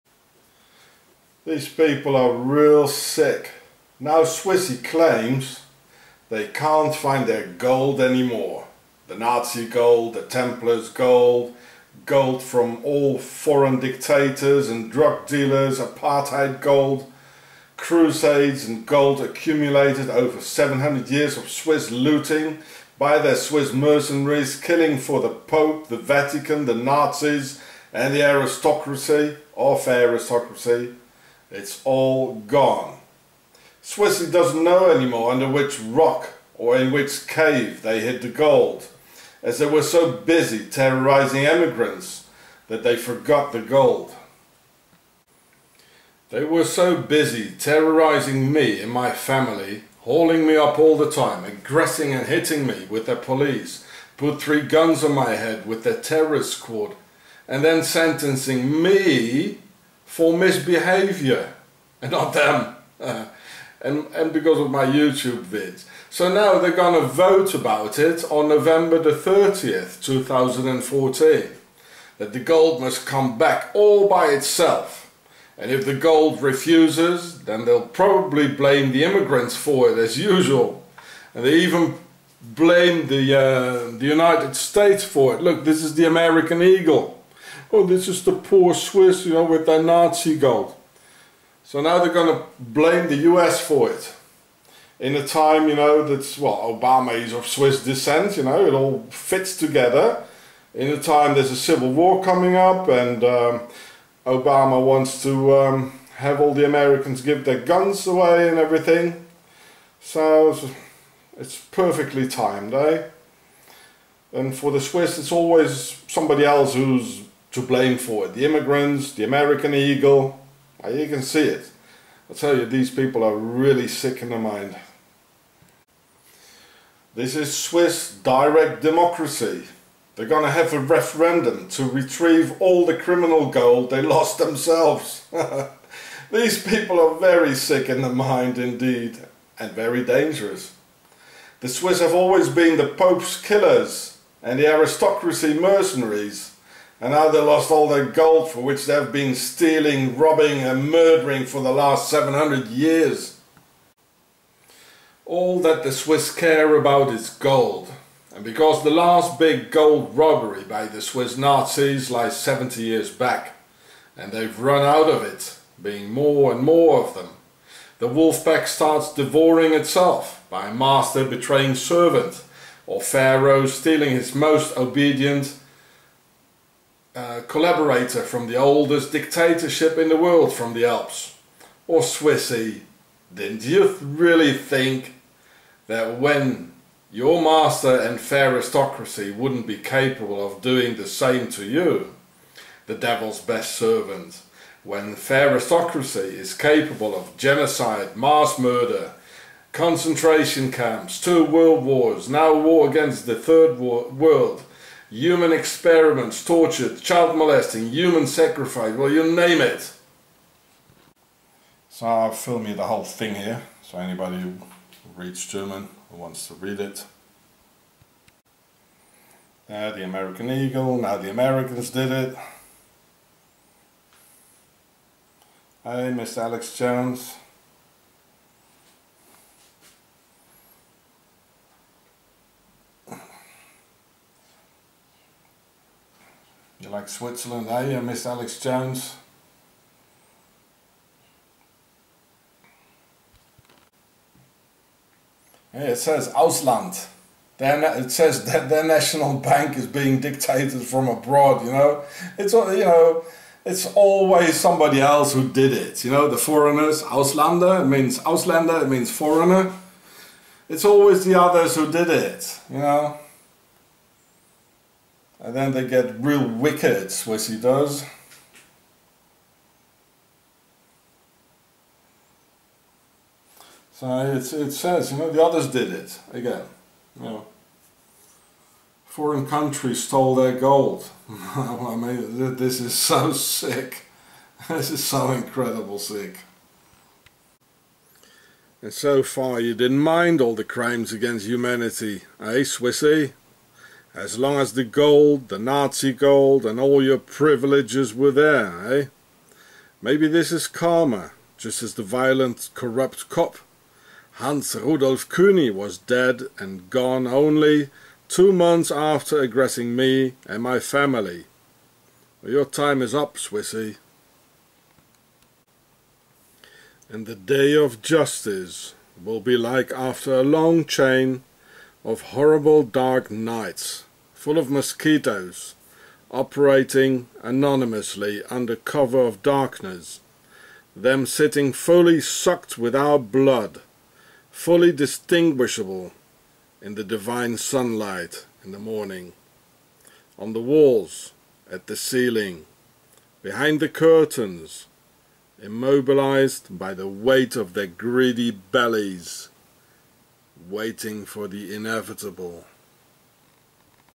Swissy can`t find Nazi Gold anymore, so they Accuse the USA in Gold-Initiative.ch
SWISSY ACCUSES THE USA, BECAUSE SWISSY CAN`T FIND THEIR OWN GOLD ANYMORE - ALL THE SWISS NAZI GOLD HAS DISAPPEARED FROM UNDER THE ROCK FROM THEIR CAVES